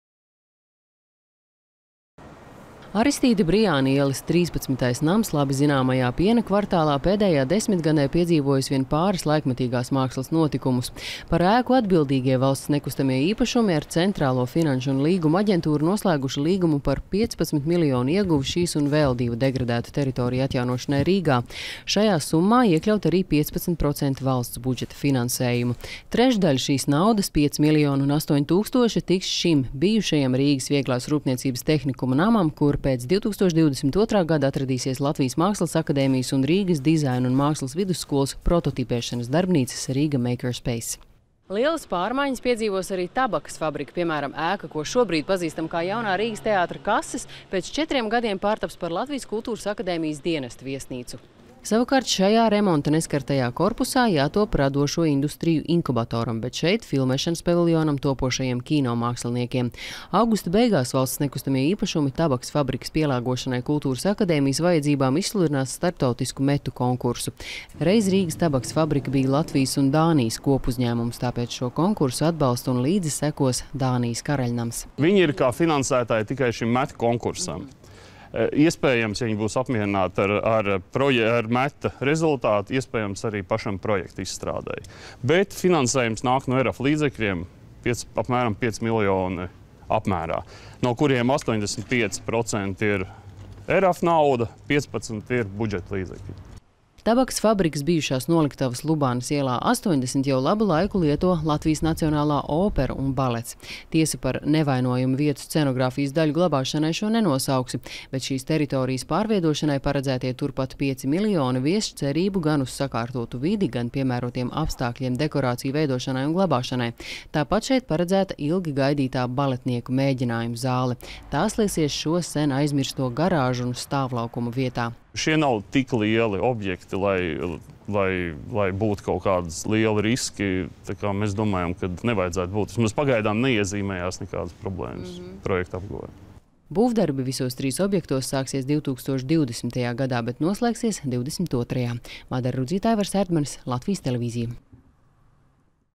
Zaļā gaisma trim kultūras infrastruktūras projektiem
Pēc dažiem gadiem Rīgā būs par trim kultūrtelpām vairāk. Bijusī Rīgas Vieglās rūpniecības tehnikuma ēka Piena kvartālā, kā arī Tabakas fabrika, kur šobrīd mīt Jaunais Rīgas teātris, un industriālā teritorija Lubānas ielā 80 tuvāko gadu laikā, piesaistot Eiropas fondu naudu, pārtaps par sakārtotiem kultūras objektiem. Par pārmaiņām ēkās, kas izdzīvošanas režīmā bijušas iekapsulētas vairākus gadu desmitus, stāsta Madara Rudzīte